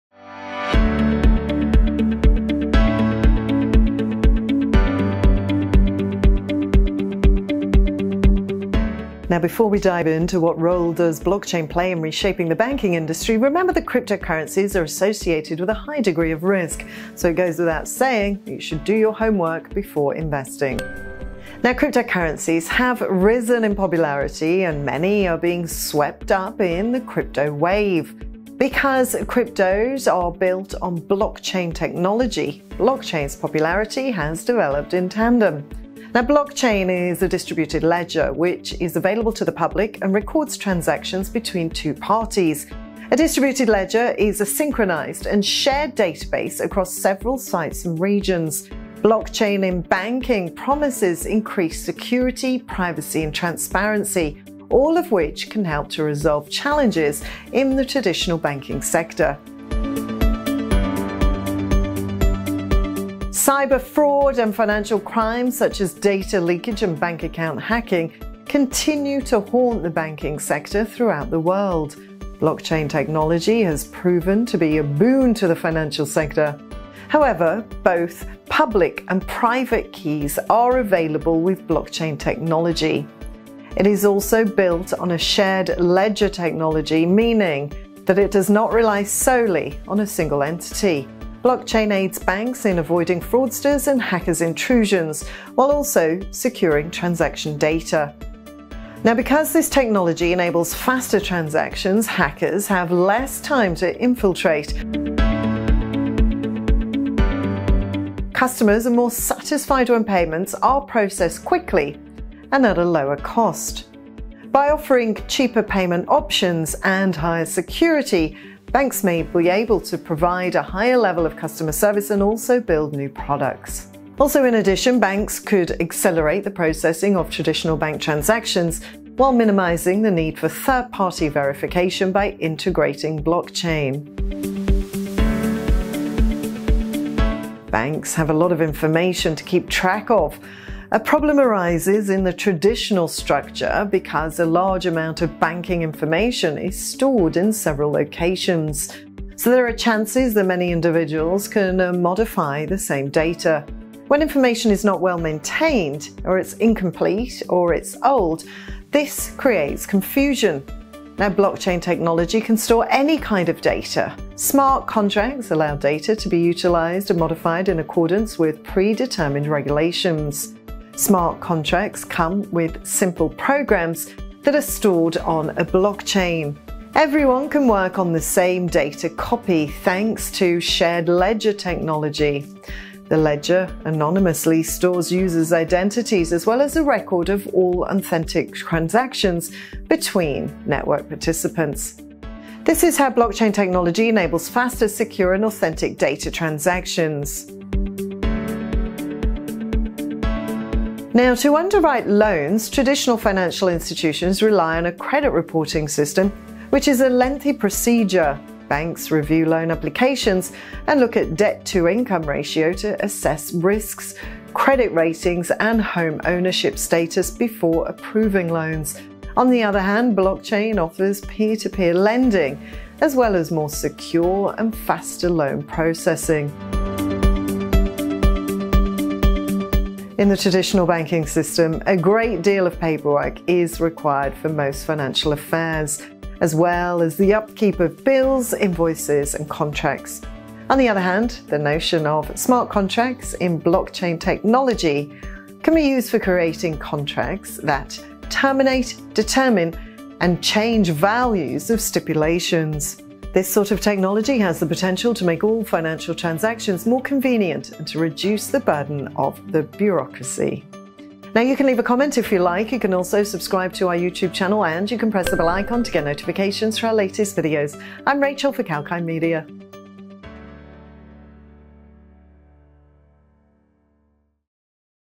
What Role Does Blockchain Play In Reshaping The Banking Industry?
Because cryptos are built on blockchain technology, blockchain’s popularity develops in tandem with crypto’s. Blockchain is a distributed ledger, which is available to the public and records transactions between two parties. Blockchain in banking promises increased security, privacy and transparency, all of which can help to resolve challenges in the traditional banking sector. Remember that cryptocurrencies are associated with a high degree of risk, so it goes without saying that you should do your homework before investing.